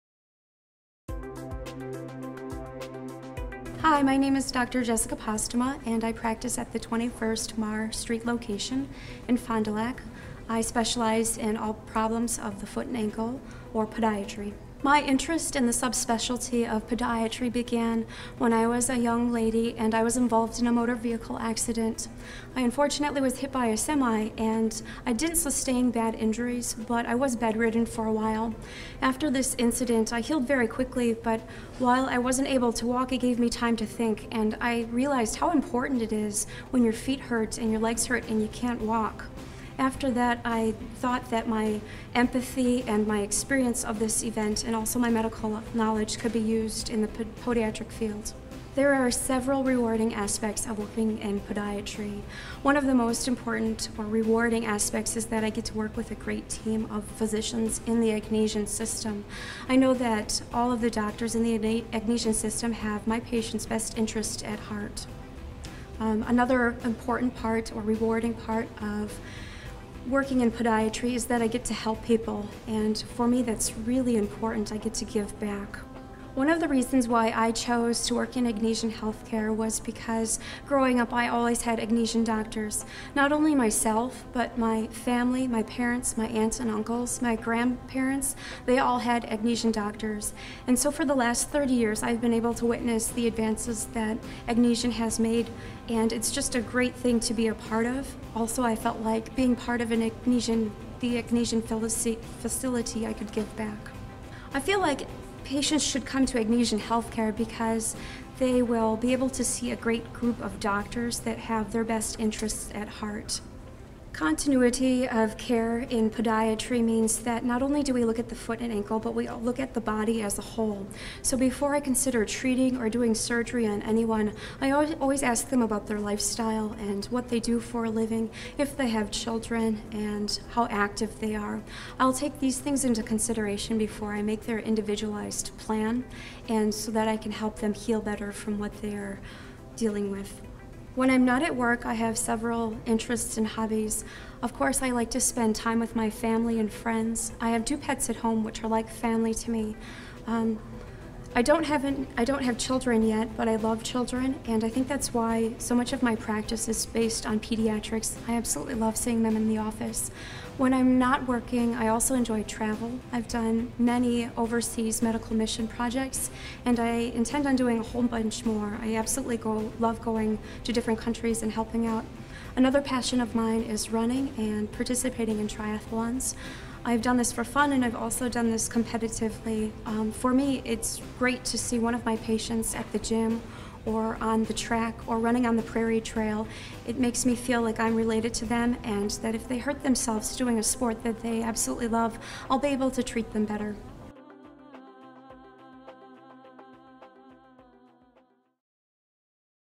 Jesika Weigel, DPM Agnesian HealthCare
Dr. Weigel graduated from the University of Wisconsin-Madison where she received a Bachelor of Science Degree in Biology.  She obtained her Doctorate from the New York College of Podiatric Medicine in 2008.  She completed a three year residency at Southeast Michigan Surgical Hospital in Warren, Michigan.  Dr. Posthuma was chief resident there during her third year. While there, she gained extensive experience in the evaluation and management of foot, ankle and lower leg pathology.  Dr. Weigel is the chair of the Wisconsin Diabetes Advisory Group for podiatric medicine.  She joined Family Foot Clinic in 2011.  She is a member of the Wisconsin Society of Podiatric Medicine and American Podiatric Medical Association. Dr. Weigel is a Wisconsin native and resides in Fond du Lac.